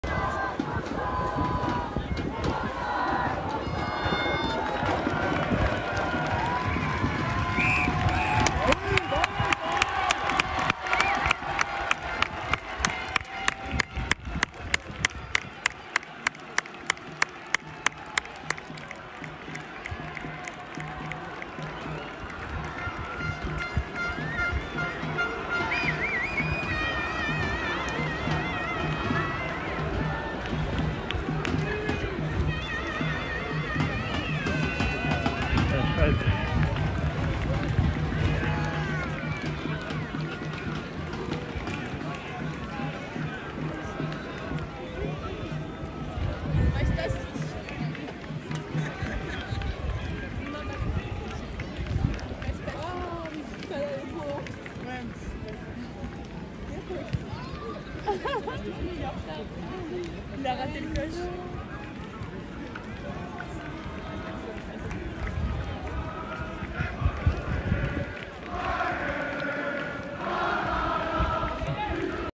12/06/2022, Montée historique du RC Versailles en Fédérale 2, communion avec le public !
Dès le coup de sifflet final du match de barrage retour face à l'autre RCV, le RC Vincennes, découvrez la réaction collective du public nombreux présent ce dimanche là à Porchefontaine.
Victoire finale 31-21 des 🔵⚪️ (48-42 au cumul des 2 matchs), qui gagnent sur le terrain la montée en division supérieure. Pour la 1ère fois de son histoire, le RCV évoluera en Fédérale 2 pour la saison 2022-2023.
Vidéo made in Lionel Ribaut @SportsCo Passion.